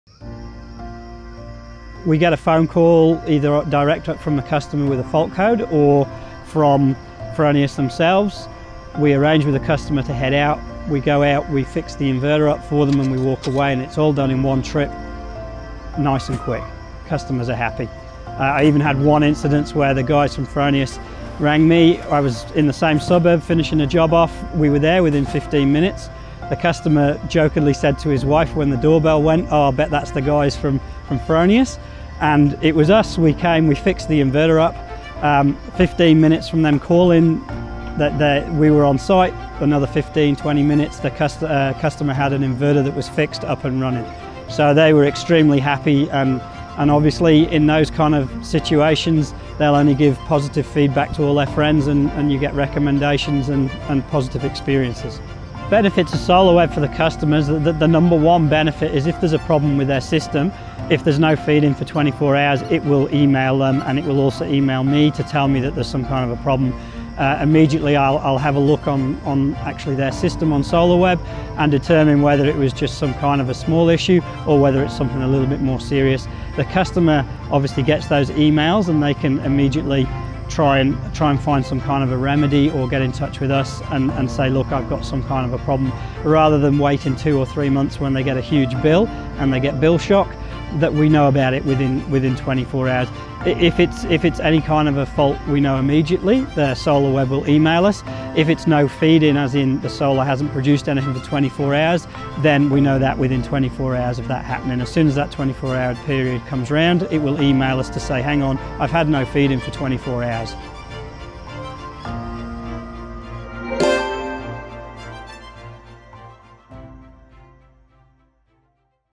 Advantages of Fronius Solar.web
Steve Lee, Director of SGL Solutions in Queensland, on the many reasons why to monitor you solar system with Fronius Solar.web.
/ Ensure functionality
/ Save more money with individual expansions
/ Expose power guzzlers
/ Extend your warranty free of charge
/ Shorten the payback period
/ Analyse and optimise energy consumption
/ Keep the system up-to-date